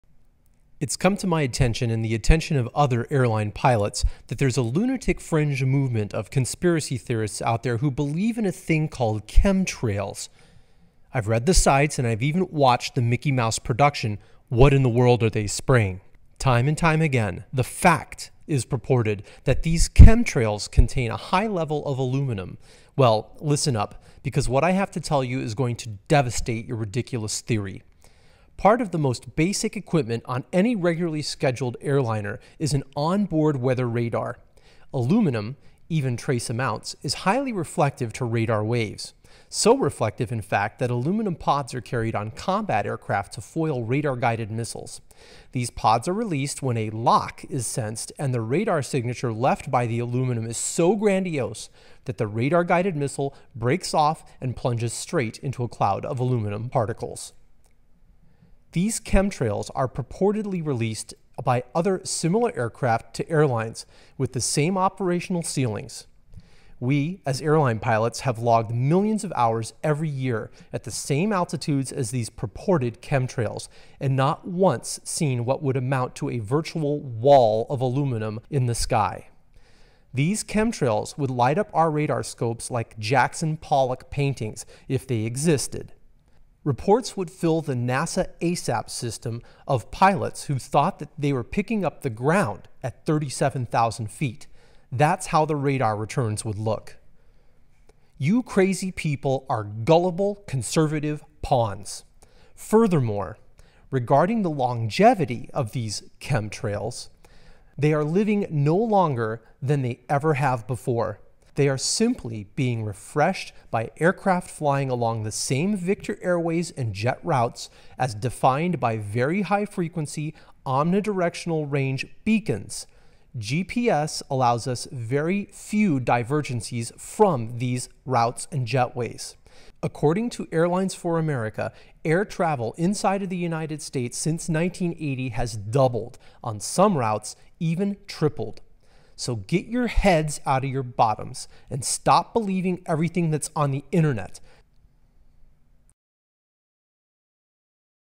Chemtrails Conspiracy Theory Completely Debunked by US Airline Pilots (Chem-trails)
Stupid American Conservative right-wing nuts debunked